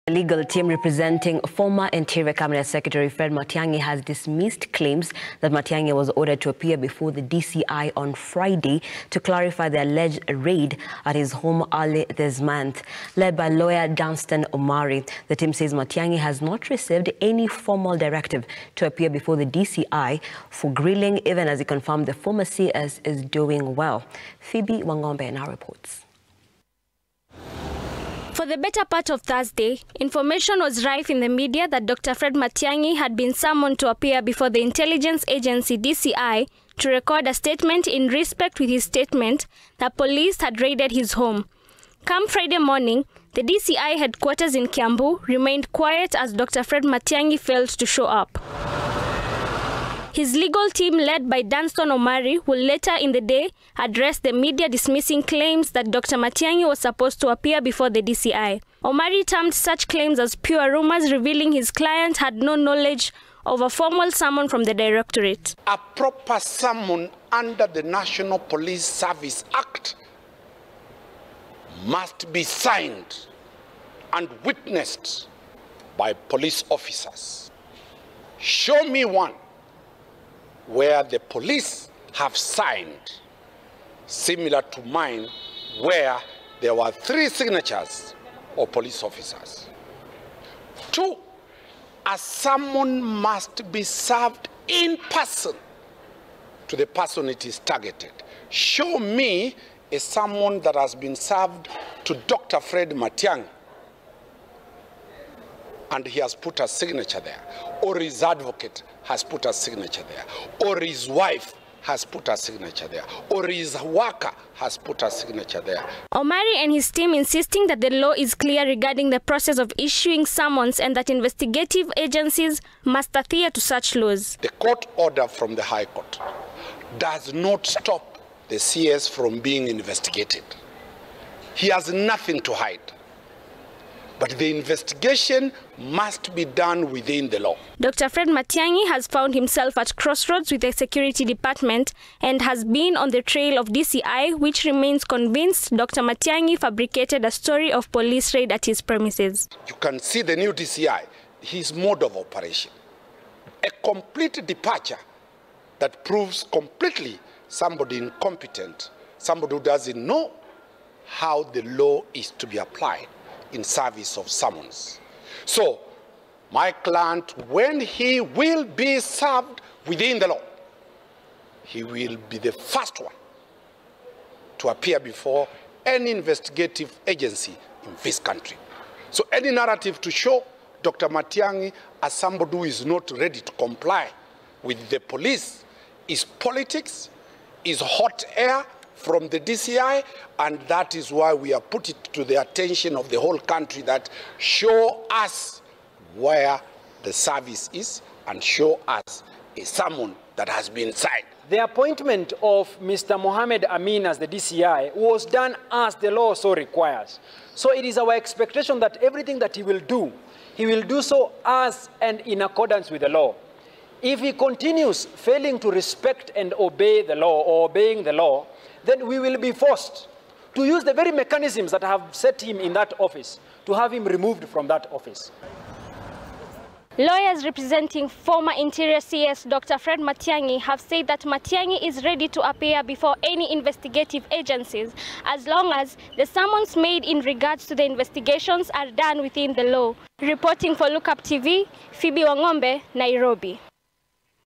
Matiangi DCI summon
The legal team representing former Interior CS Dr Fred Matiang'i has dismissed claims that Matiangi was ordered to appear before the DCIon Friday to clarify the alleged raid at his home early this month.
Led by lawyer Danstan Omari, the team says, Matiangi has not received any formal directive to appear before the dci for grilling even as he confirmed the former CS is doing well.